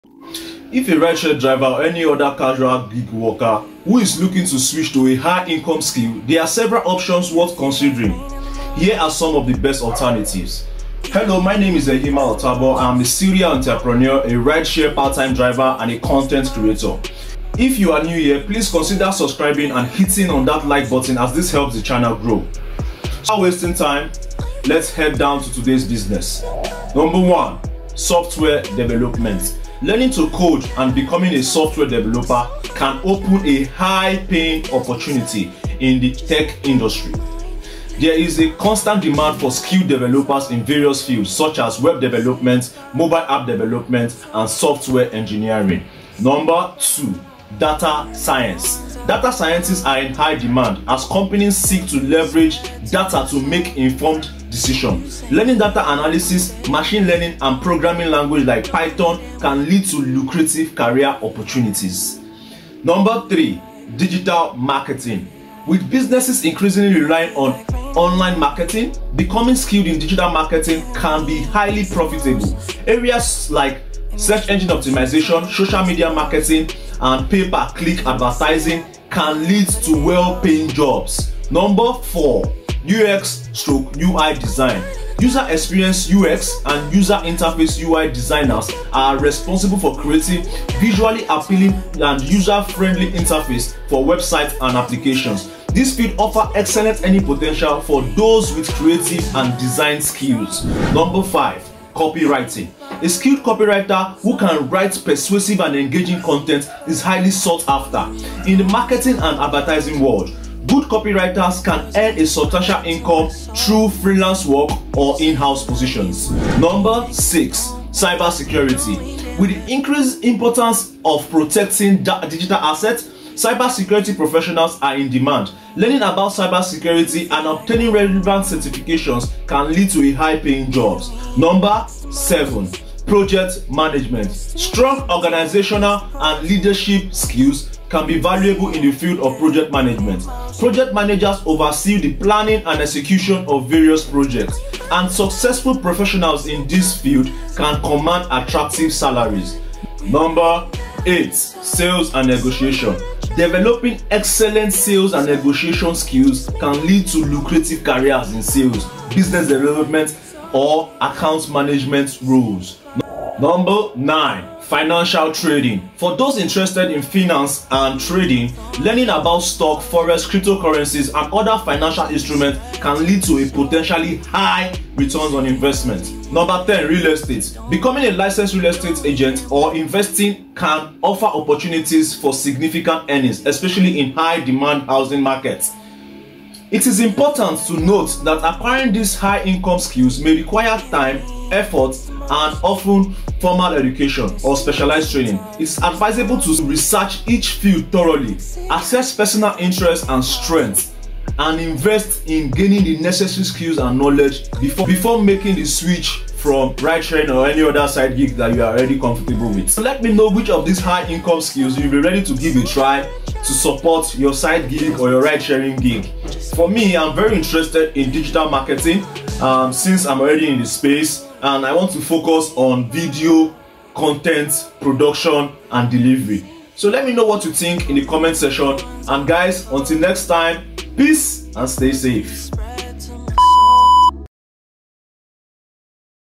REPLACE RIDESHARE WITH ANY OF THESE HIGH-INCOME SKILLS IN 2023
If a rideshare driver is looking to switch to a high-income skill, there are several options worth considering. Here are some of the best alternatives:

1. Software Development: Learning how to code and becoming a software developer can open up high-paying opportunities in the tech industry. There is a constant demand for skilled developers in various fields, such as web development, mobile app development, and software engineering.

2. Data Science: Data scientists are in high demand as companies seek to leverage data to make informed decisions. Learning data analysis, machine learning, and programming languages like Python can lead to lucrative career opportunities.

3. Digital Marketing: With businesses increasingly relying on online marketing, becoming skilled in digital marketing can be highly profitable. Areas like search engine optimization (SEO), social media marketing, and pay-per-click (PPC) advertising can lead to well-paying jobs.

4. UX/UI Design: User experience (UX) and user interface (UI) designers are responsible for creating visually appealing and user-friendly interfaces for websites and applications. This field offers excellent earning potential for those with creative and design skills.

5. Copywriting: A skilled copywriter who can write persuasive and engaging content is highly sought after in the marketing and advertising world. Good copywriters can earn a substantial income through freelance work or in-house positions.

6. Cybersecurity: With the increasing importance of protecting digital assets, cybersecurity professionals are in demand. Learning about cybersecurity and obtaining relevant certifications can lead to high-paying jobs.

7. Project Management: Strong organizational and leadership skills can be valuable in the field of project management. Project managers oversee the planning and execution of various projects, and successful professionals in this field can command attractive salaries.

8. Sales and Negotiation: Developing excellent sales and negotiation skills can lead to lucrative careers in sales, business development, or account management roles.

9. Financial Trading: For those interested in finance and trading, learning about stocks, forex, cryptocurrencies, and other financial instruments can lead to potentially high returns on investments.

10. Real Estate: Becoming a licensed real estate agent or investor can offer opportunities for significant earnings, especially in high-demand housing markets.

It's important to note that acquiring these high-income skills may require time, effort, and often formal education or specialized training. It's advisable to research each field thoroughly, assess personal interests and strengths, and invest in gaining the necessary skills and knowledge before making the switch from ridesharing.